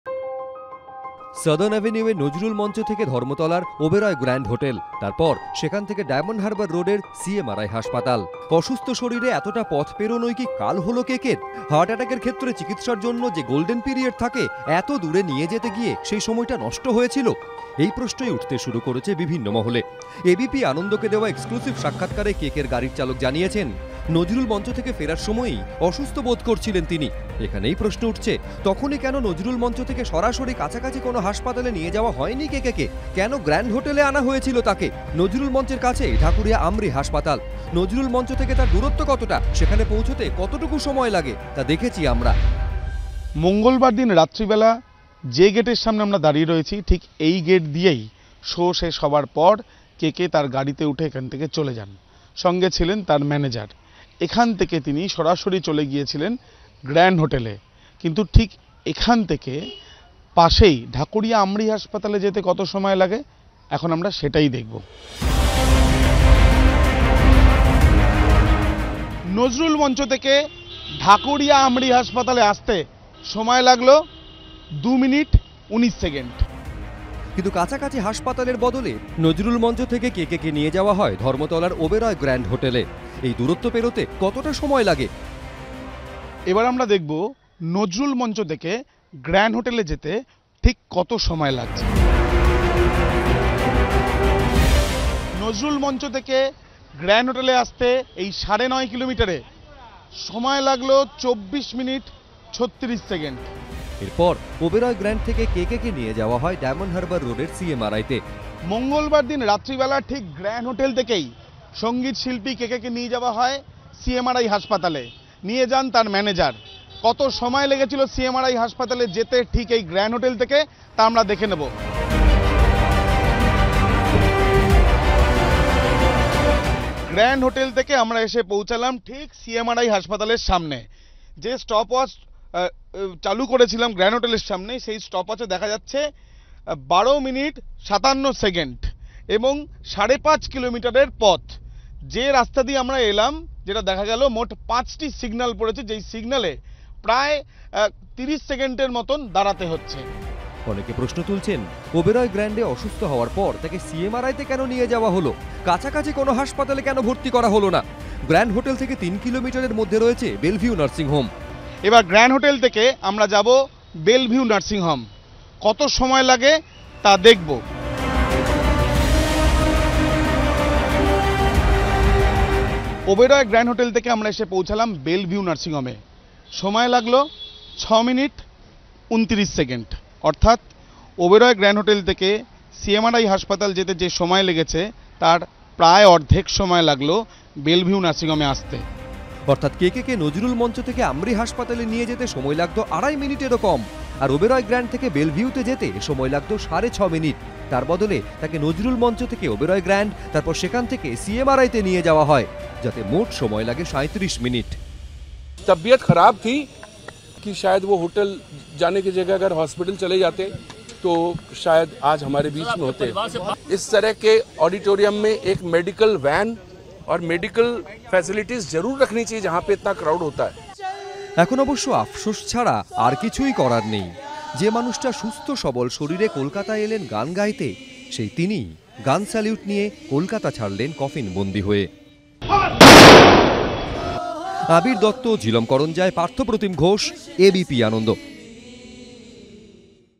KK কেকে অসুস্থ হয়ে পড়ার পর, তাঁকে দ্রুত কাছের কোনও হাসপাতালে নিয়ে যাওয়া হল না কেন ?
কেকে অসুস্থ হয়ে পড়ার পর, তাঁকে দ্রুত কাছের কোনও হাসপাতালে নিয়ে যাওয়া হল না কেন? প্রথমে ওবেরয় গ্র্যান্ড হোটেল, তারপর সেখান থেকে CMRI’তে নিয়ে যাওয়া হল কেন? এই গোল্ডেন পিরিয়ড নষ্ট না হলে, কি কেকে’কে বাঁচানো যেত? সেই প্রশ্ন এখন ঘুরপাক খাচ্ছে বিভিন্ন মহলে।